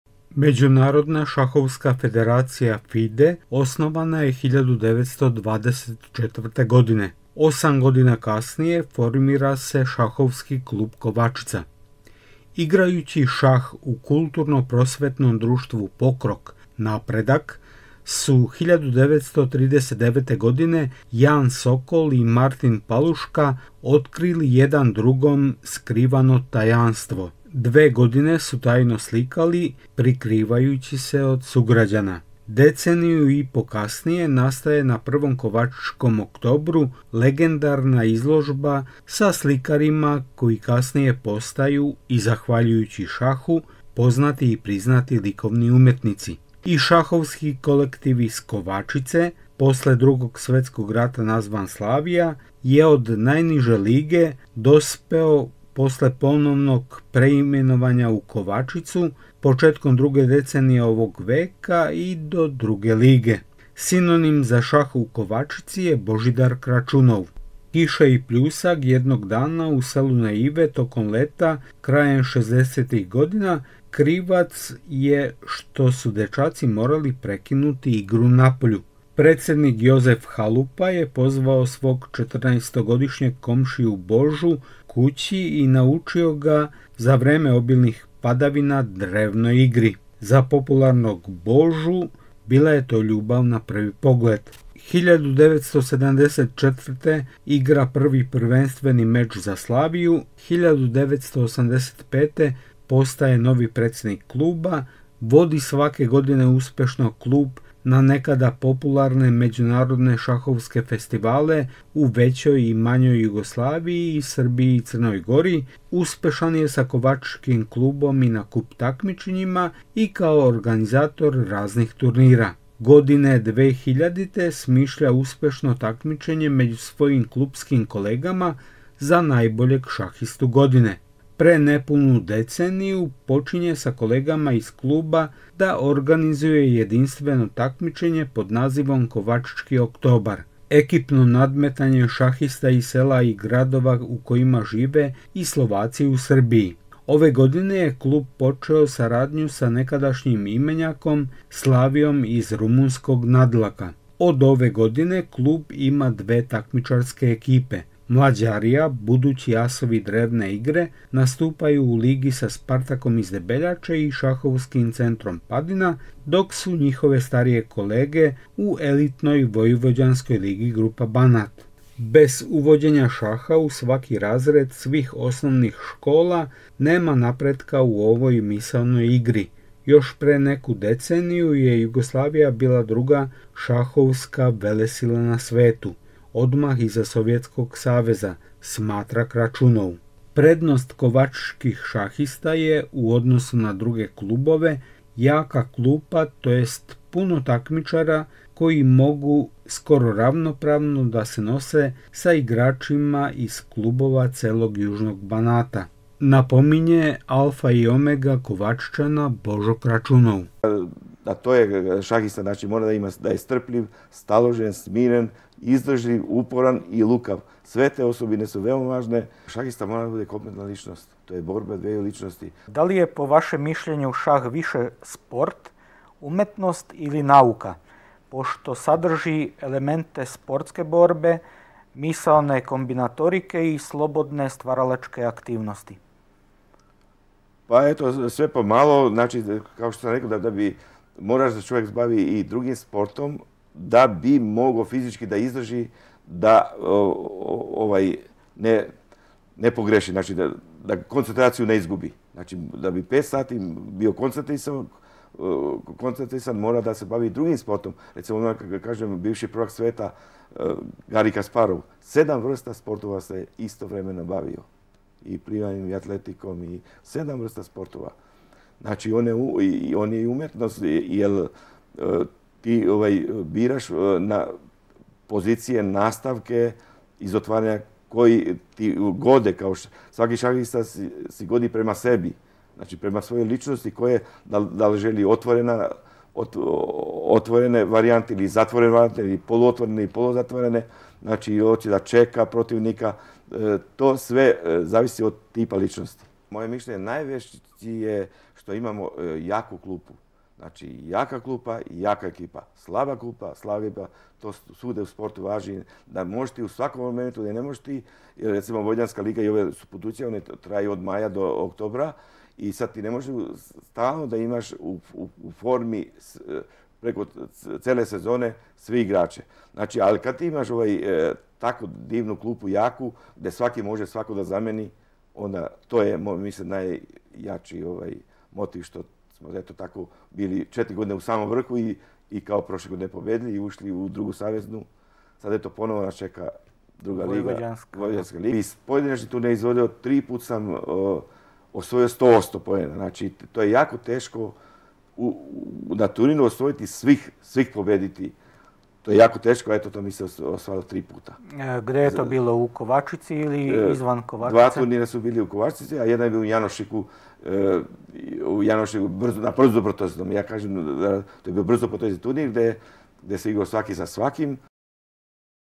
Portret Božidara Kračunova
Medjunarodna šahovska federacija – FIDE – osnovana je u Parizu 1924. godine. Osam godina kasnije formira se Šahovski klub "Kovačica". Igrajući šah u Kulturno-prosvetnom društvu „Pokrok“ (u prevodu "Napredak“) su 1939. godine Jan Sokol i Martin Paluška otkrili jedan drugom skrivano tajanstvo: dve godine su tajno slikali prikrivajući se od sugradjana. Deceniju i po kasnije nastaje na prvom Kovačičkom oktobru legendarna izložba sa slikarima koji kasnije postaju – i zahvaljujući šahu – poznati i priznati likovni umetnici. I šahovski kolektiv iz Kovačice – posle drugog svetskog rata nazvan Slavija – je od najniže lige dospeo posle ponovnog preimenovanja u Kovačicu - početkom druge decenije ovog veka i do Druge lige. Sinonim za šah u Kovačici je Božidar Kračunov.